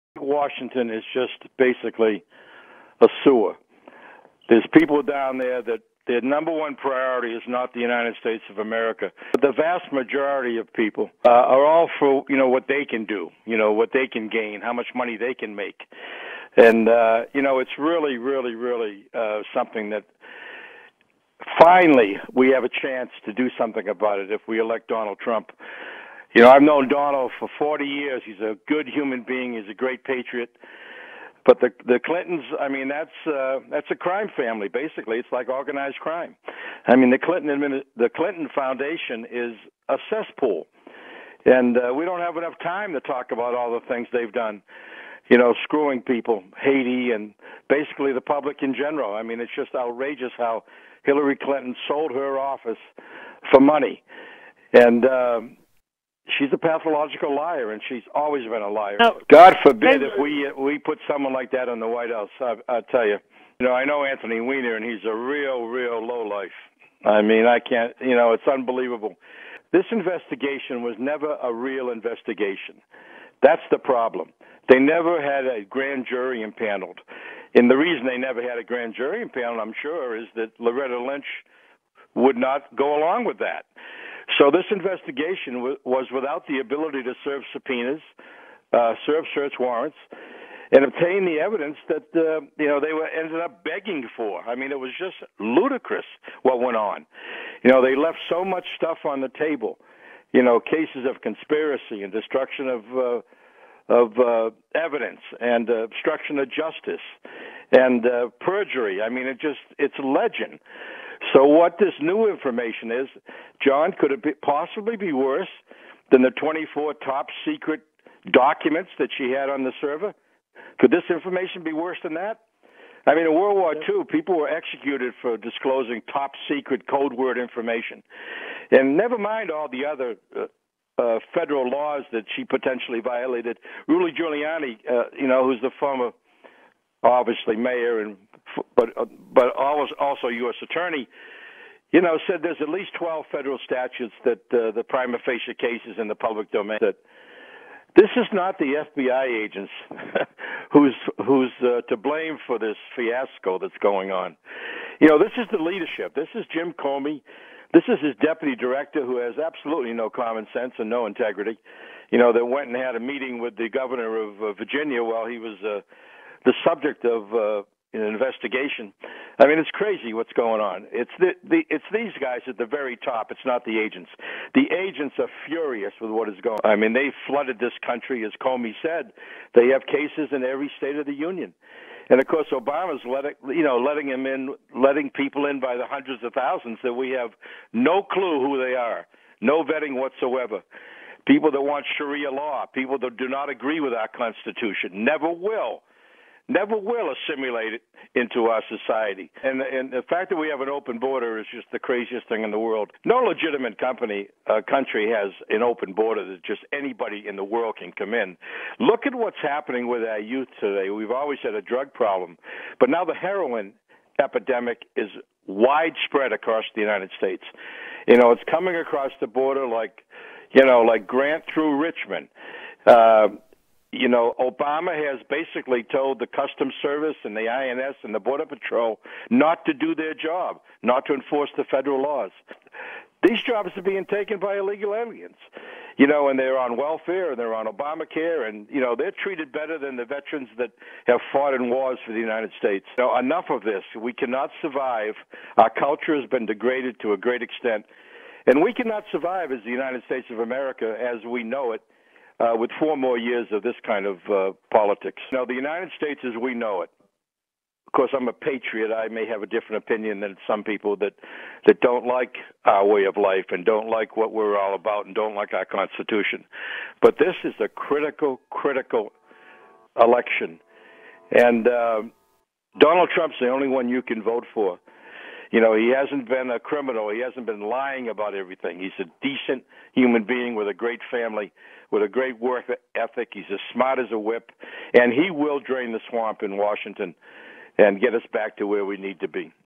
James Kallstrom (FBI ret) says Clintons are a crime family (full audio, edited for time)
Former Assistant Director of the FBI's New York Division says our country is at stake in this election. Clinton Crimes too numerous to list. Our culture has been degraded to a great extent. Muslim Terrorists being brought into the country via Obama's refugee programs. Character references Donald Trump

RealityCheckRadio.net